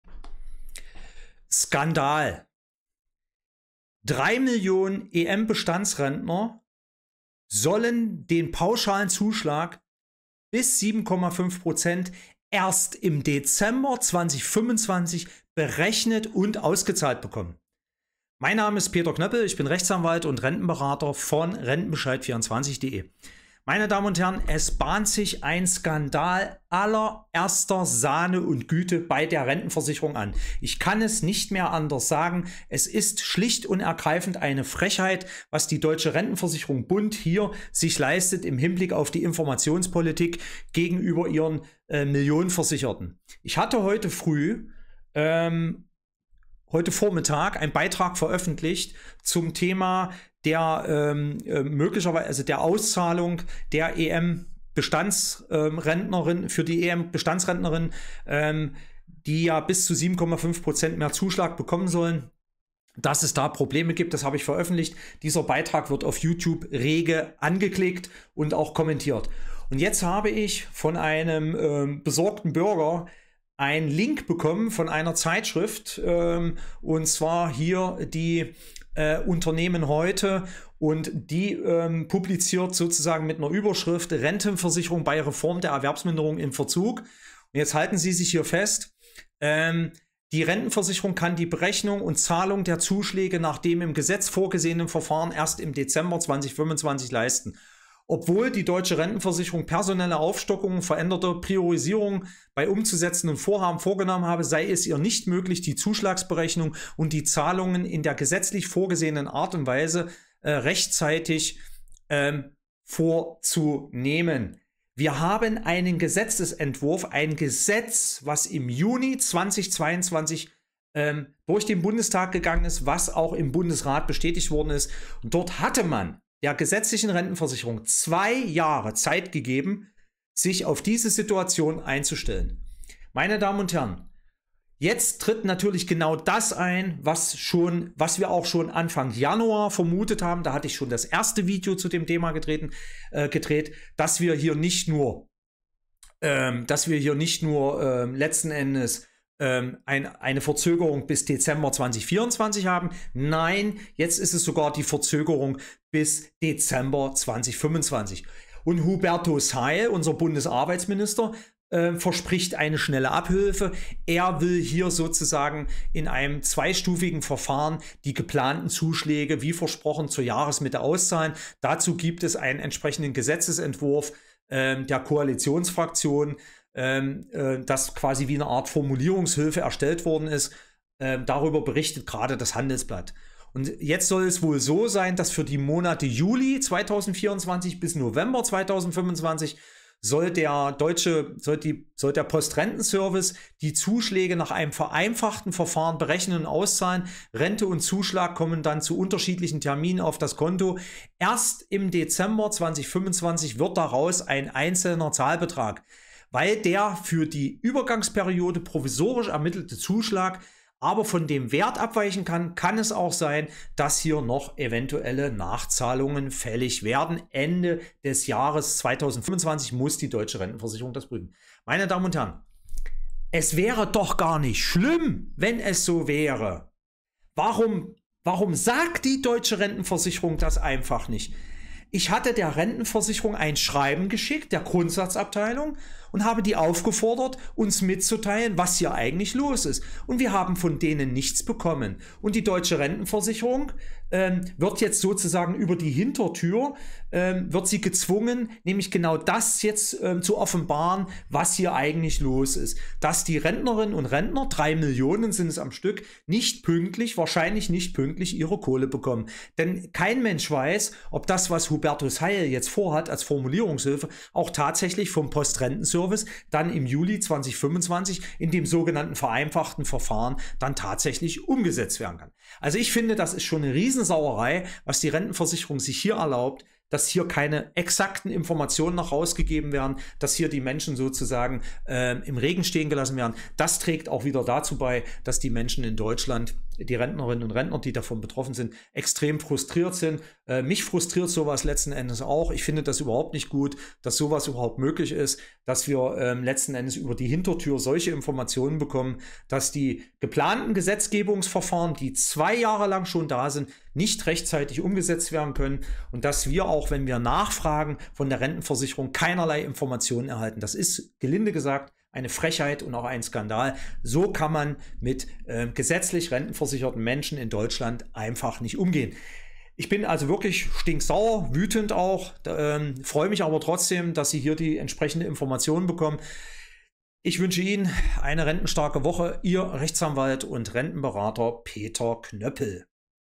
Bodenlose Frechheit: EM-Rentenzuschlag bis 7,5% erst Ende 2025!
Unglaubliches was ich heute am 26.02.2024 berichten muss. Die Deutsche Rentenversicherung schafft es nicht den Zuschlag für 3 Millionen EM-Rentner pünktlich zum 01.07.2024 auszuzahlen. Die Berechnung und Auszahlung kann wohl erst Ende 2025 erfolgen. Davon berichtete am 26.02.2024 die Online Zeitung Unternehmen-Heute unter Verweis auf das Handelsblatt.
Es ist gelinde gesagt ein Skandal und eine Frechheit dass die deutsche Rentenversicherung über dieses Problem mit der Umsetzung nicht den Kunden mitteilt. 3 Millionen Versicherte warten seit 2 Jahren auf diesen Rentenzuschlag.  Nun soll es diesen erst Ende 2025 geben? Hubertus Heil Bundesminister für Arbeit und Soziales verspricht Abhilfe. Er will über ein zweistufiges Verfahren die Auszahlung in der Zeit vom Juli 2024 bis November 2025 über den Postrentenservice organisieren. Hierzu muss aber eine Rechtsänderungen im Wege eines Gesetzgebungsverfahrens beschlossen werden.  Insoweit bleibt abzuwarten, ob dies dem Minister gelingt.
Rentenberater Peter Knöppel sagt. Es ist unglaublich und skandalös, dass die Rentenversicherung hier die Menschen im "Regen" stehen lässt und die pünktliche Auszahlung des Rentenzuschlages nicht ordnungsgemäß erfolgen kann.